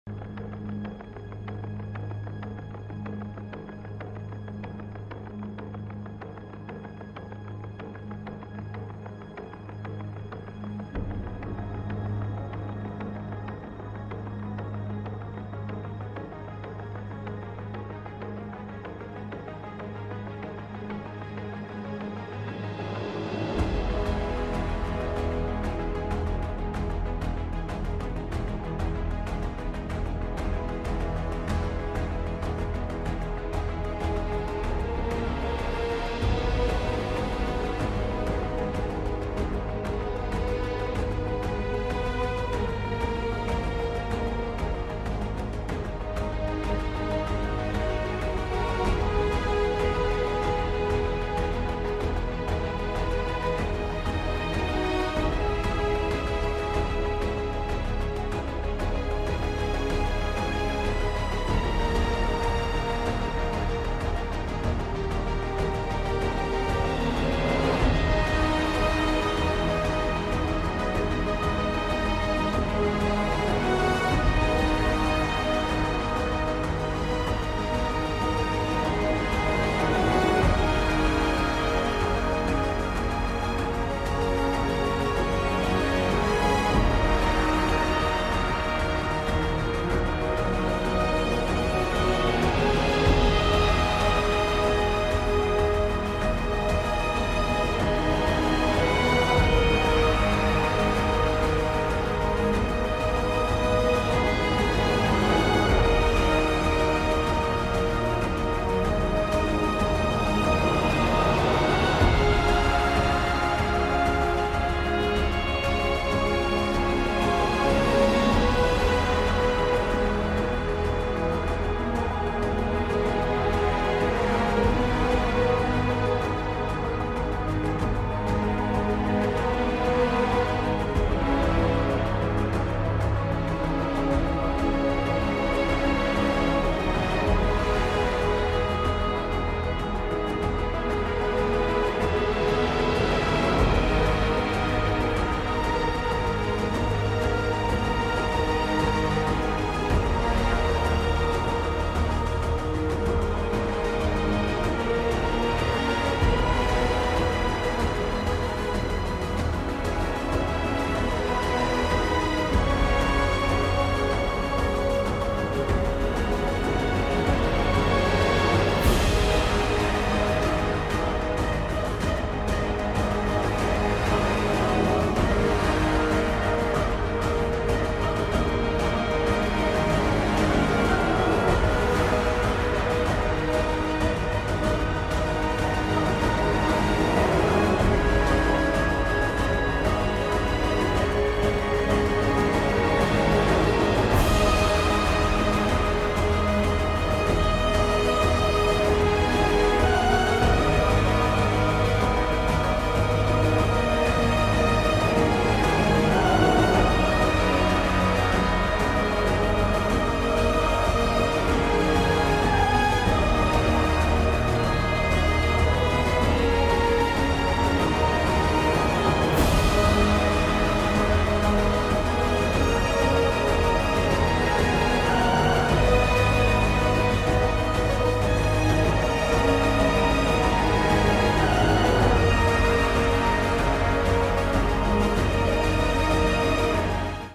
Regresaron las noches mágicas al estadio Juan Francisco Barraza
DESPUÉS DE 5 AÑOS SAN MIGUEL VOLVIÓ A DISFRUTAR UN PARTIDO DE FUTBOL DE NOCHE EN EL ESTADIO BARRAZA Y QUE MEJOR FORMAR DE HACERLO ENTRE CD ÁGUILA VS CD FAS